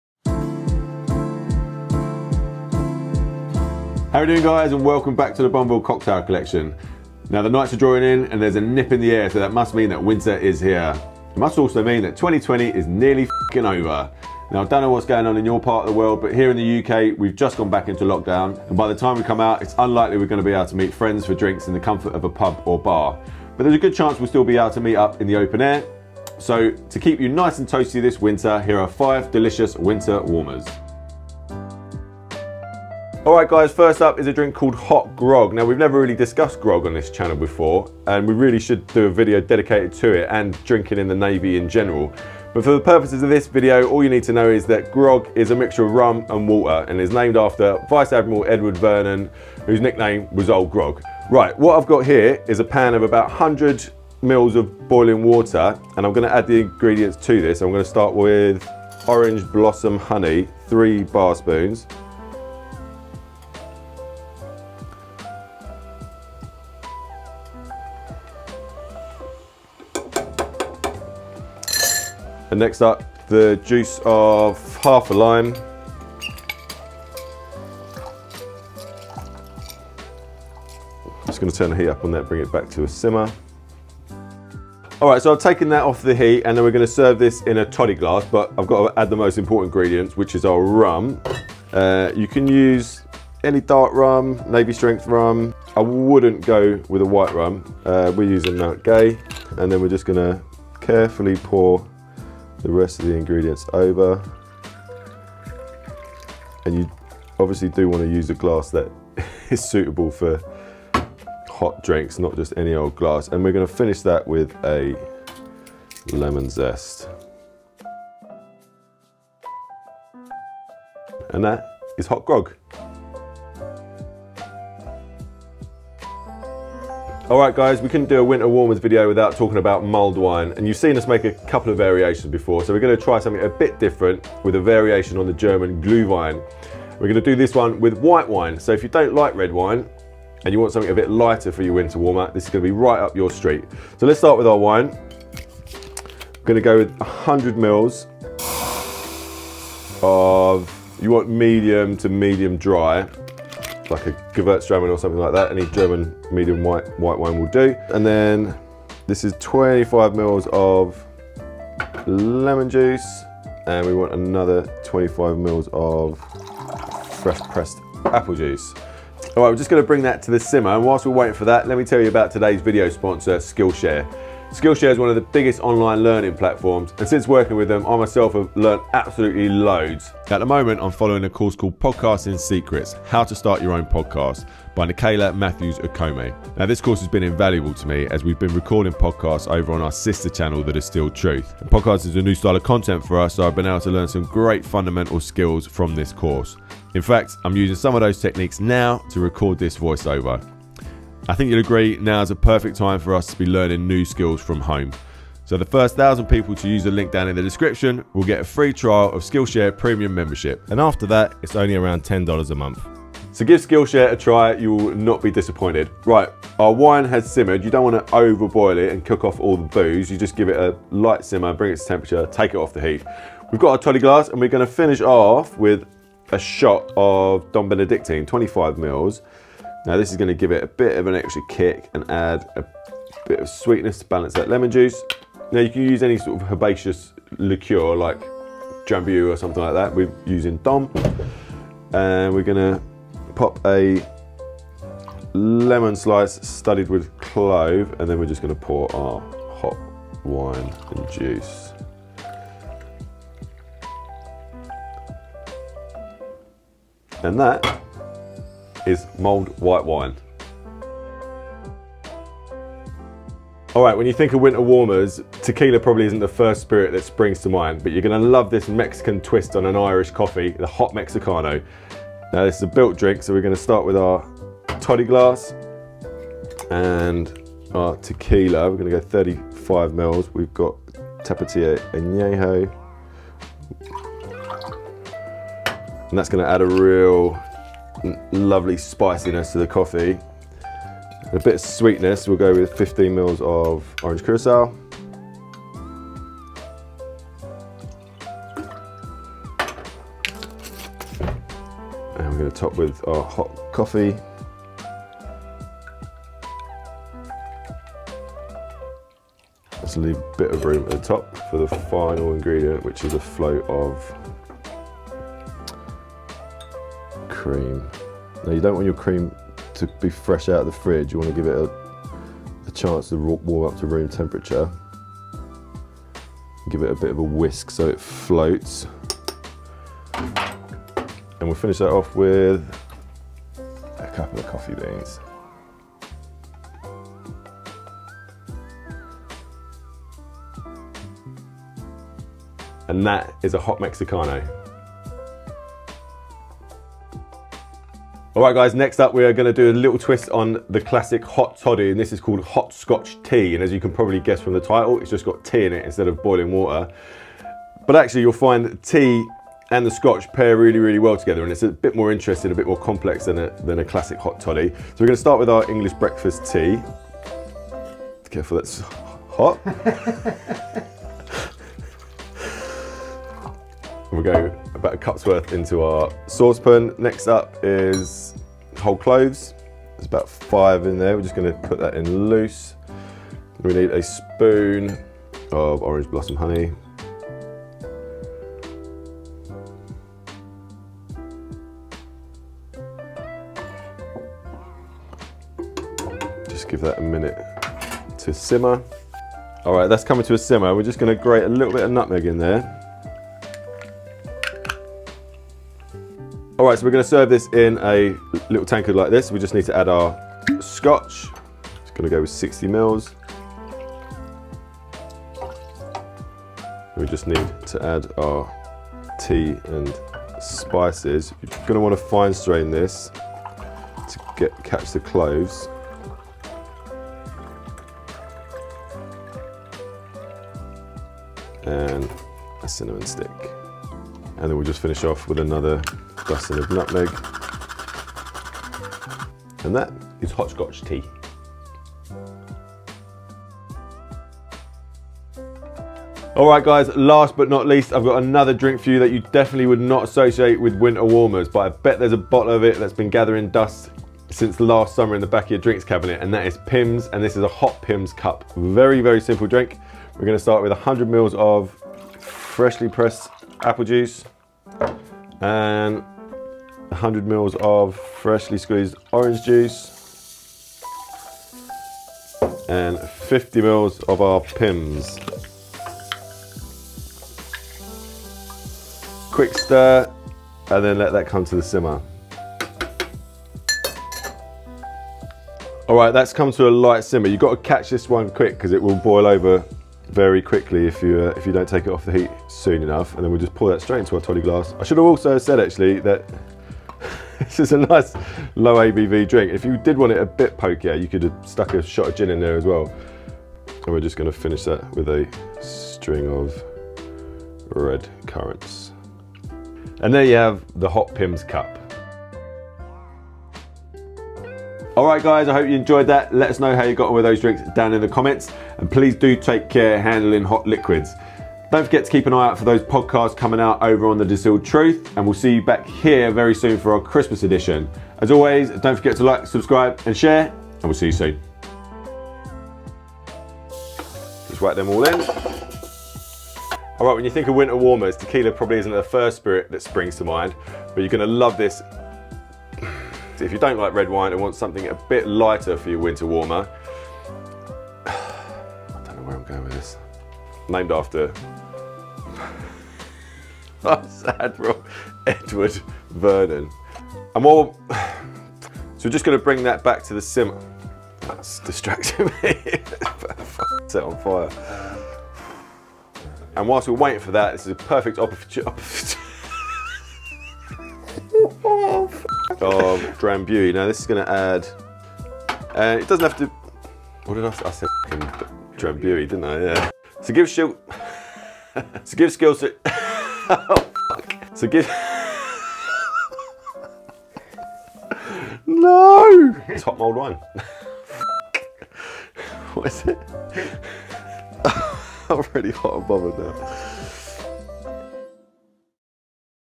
5 Winter Warmer Cocktails
TIMESTAMPS
0:00 - INTRO
0:31 - HOT GROG
2:06 - MULLED WHITE WINE
4:22 - HOT MEXICANO
5:48 - HOT SCOTCH TEA
7:23 - HOT PIMMS CUP
8:53 - OUTTAKES!

RECOMMENDED READING

STARTER COCKTAIL SET WE LIKE